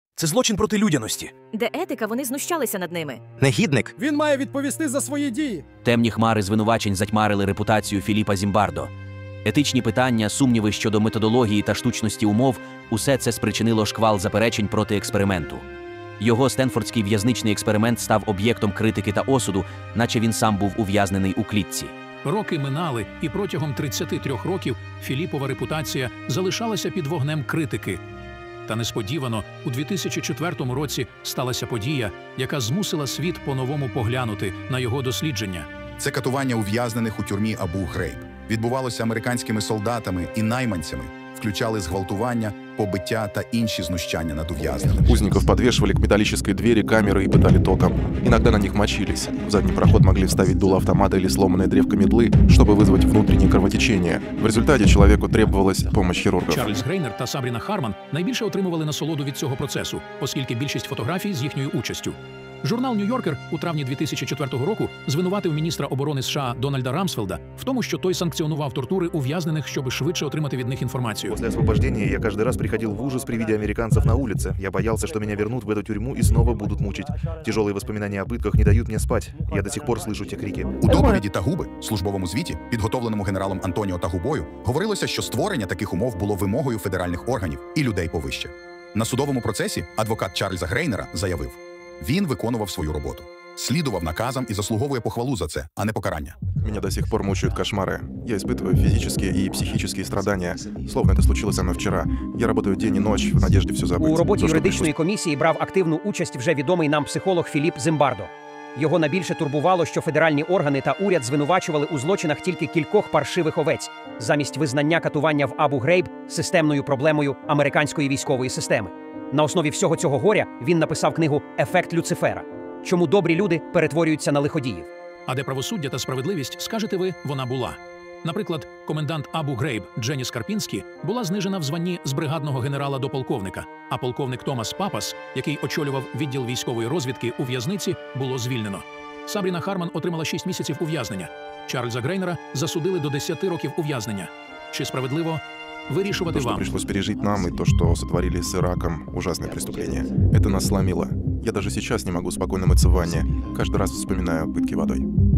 Катування ув'язнених у тюрмі Абу-Грейб. Ефект Люцифера. Чому добрі люди перетворюються на злодіїв?
Протягом десятиліть Стенфордський в'язничний експеримент Філіпа Зімбардо був об'єктом безжальної критики та етичних звинувачень. Його методологія, умови проведення - все було поставлено під сумнів.

Проте у 2004 році сталася трагічна подія, яка змусила світ по-новому поглянути на це скандальне дослідження. Жорстоке поводження американських військових з ув'язненими в Абу-Грейб стало реальним втіленням моделі, відтвореної Зімбардо.

У новому відео ми розглянемо, як події в іракській в'язниці виправдали піддані осуду експерименти. Чи була насправді штучна ситуація такою вже надуманою? Приєднуйтесь!

Відео створене за допомогою штучного інтелекту. Використовувалась фонова музика - , Clipdrop - генератор зображень, genmo - генератор відео, leonardoai - генератор зображення, LeiaPix - створює анімації, kaiber - генератор відео, озвучка за допомогою- Clipchamp , Canva - відеоредактор та Gpt + Bard.